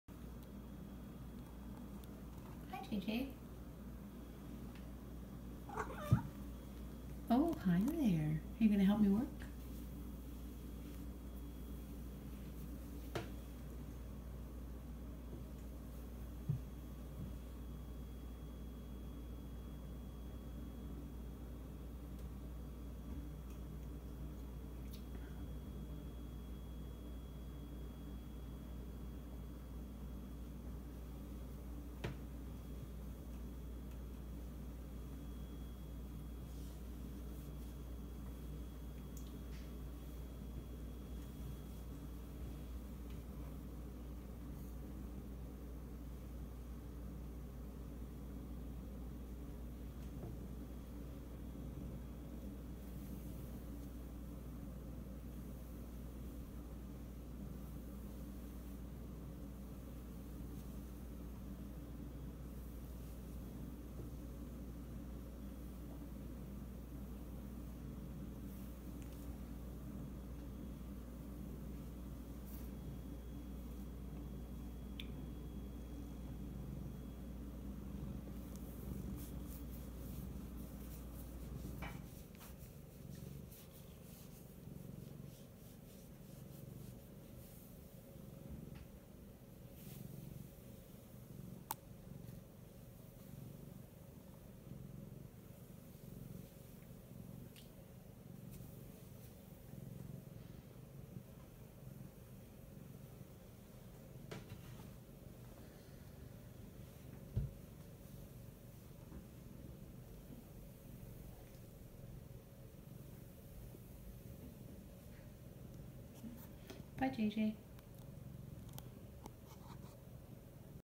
JJ Decided My Lap is a Good Place to Be
JJ is a torontocatrescue cat. Just yesterday I told a potential adopter that he didn't really like to sit on peoples laps… So of course today he has been sitting on my lap lots! He hasn't stayed for too long, but that will probably change now that I've said it!